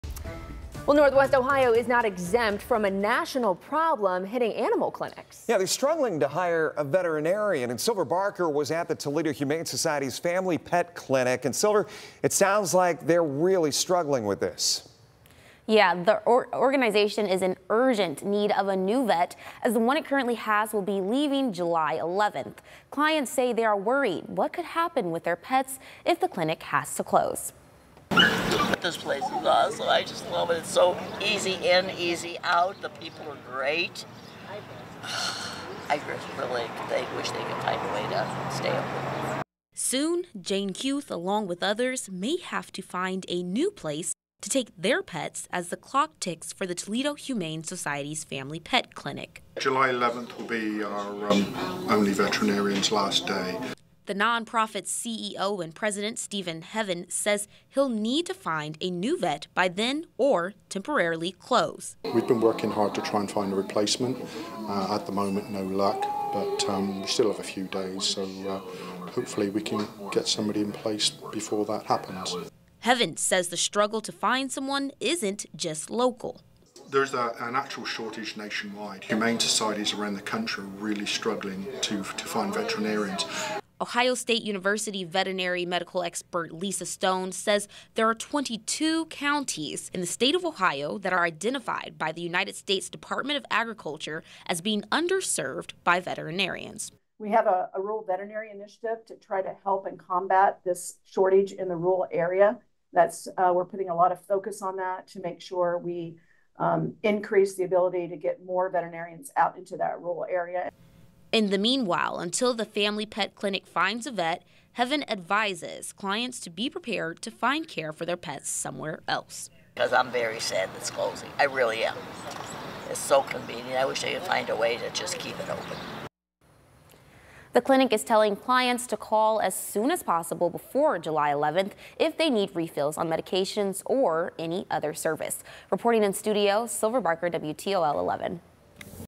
Toledo Humane Society Family Pet Clinic may temporarily close if new veterinarian isn't found
The Toledo Humane Society Family Pet Clinic is in urgent need of a new veterinarian as its current one is leaving July 11.